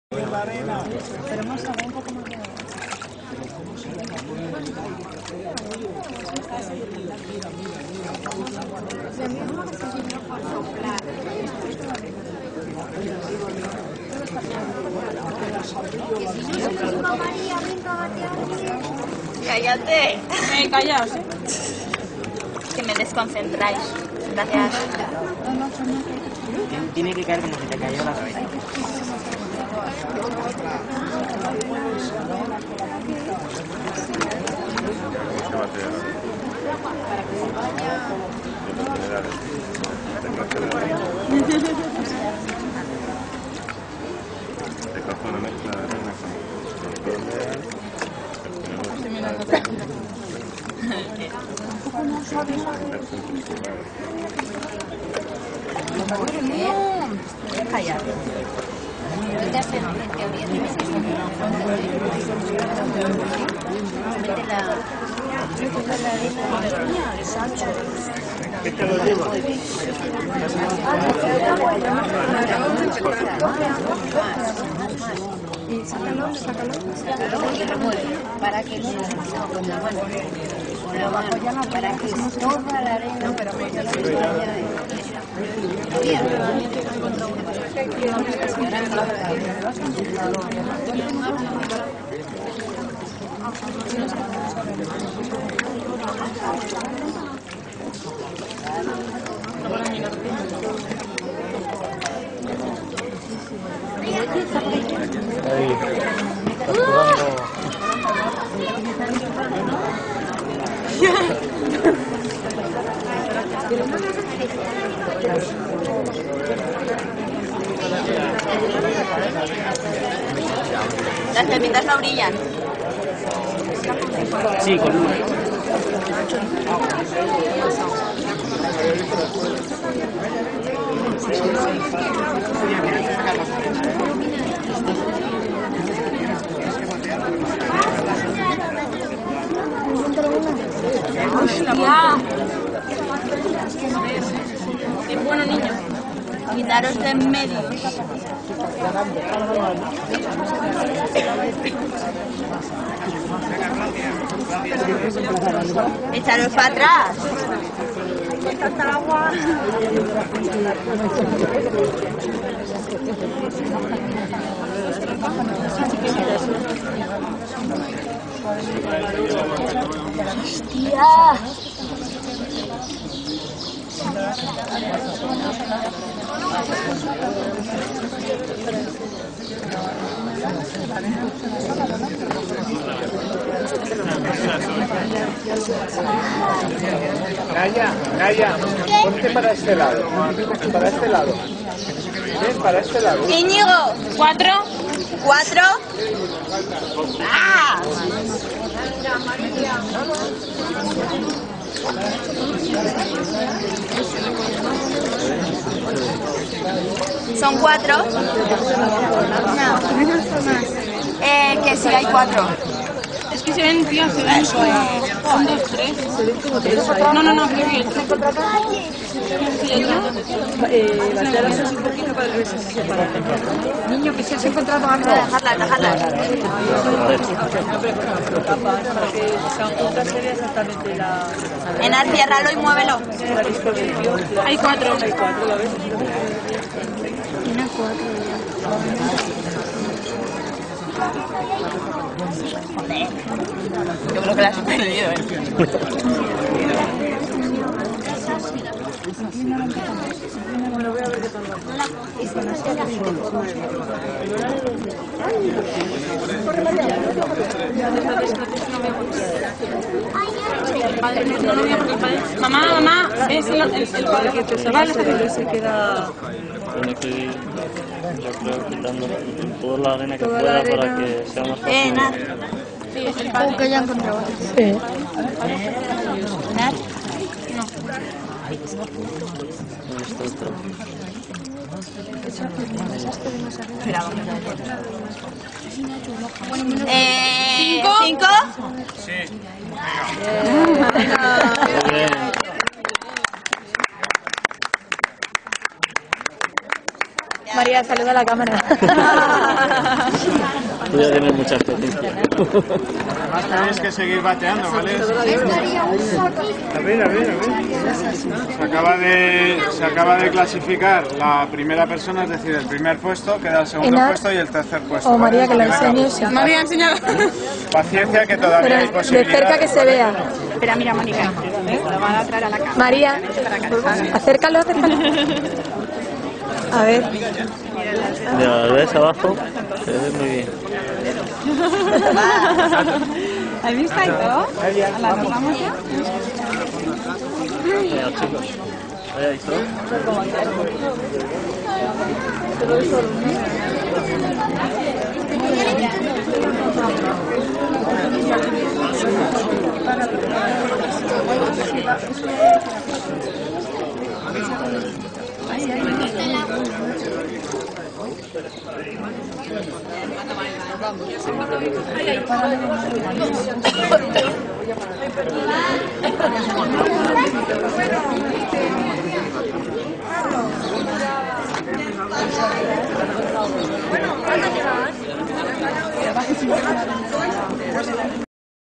Final Concurso de Bateo de oro Expominerales 2009
Final del concurso de bateo de oro de la XXV edición de la Feria Expominerales, celebrada del 6 al 8 de marzo en la E.T.S.I. de Minas de la Universidad Politécnica de Madrid.